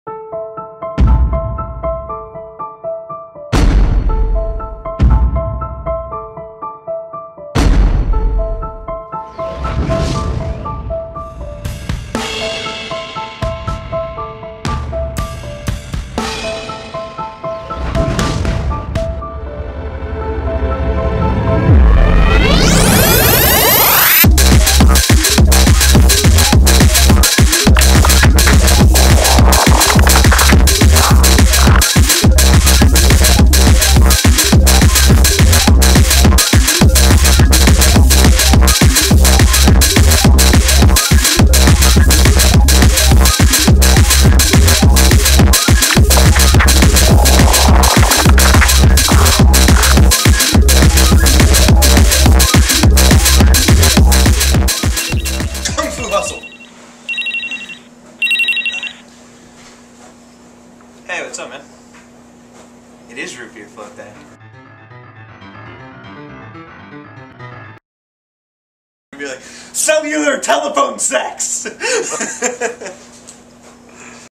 Root Beer Float Maniac
August 6th, 2011 was National Root Beer Float Day. We decided to make some to celebrate. But Matt got a little out of hand. This is what happened that night.